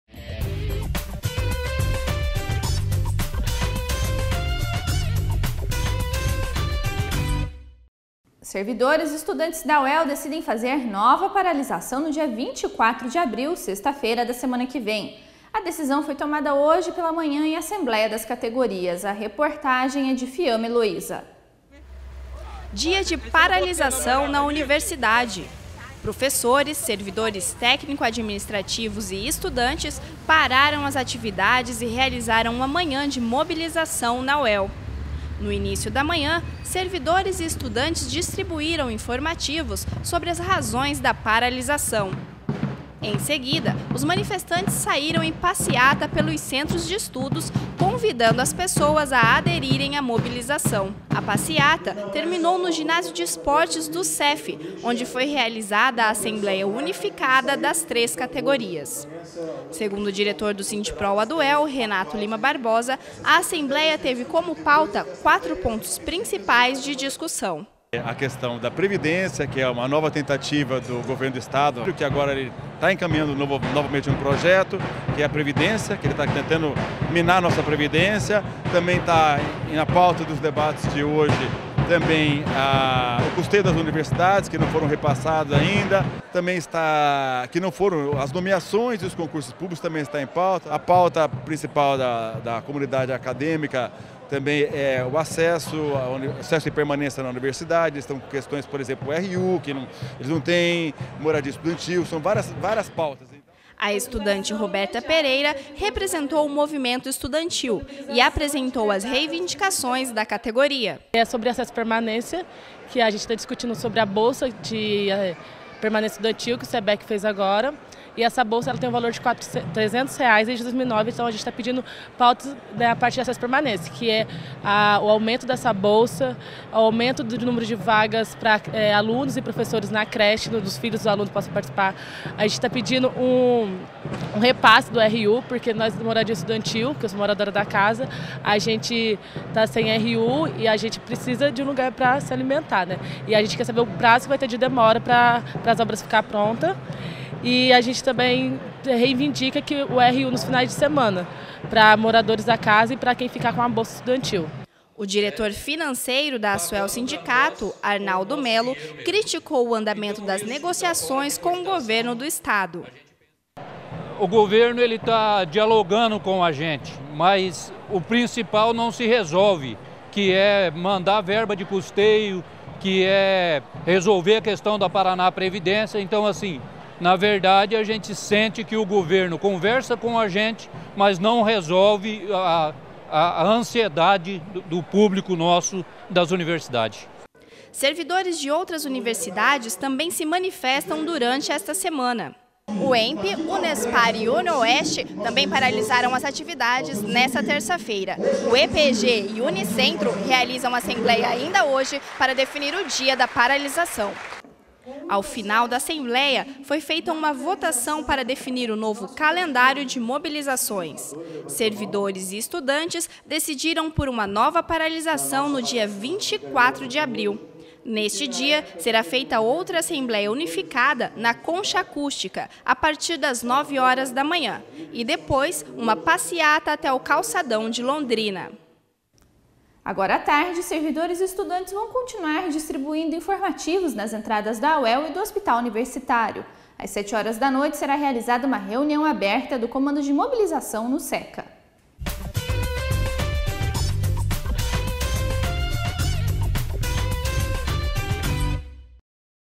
Servidores e estudantes da UEL decidem fazer nova paralisação
A nova paralisação será no dia 24 de abril, sexta-feira da semana que vem. A decisão foi tomada hoje pela manhã em assembleia unificada de professores, servidores técnico-administrativos e estudantes.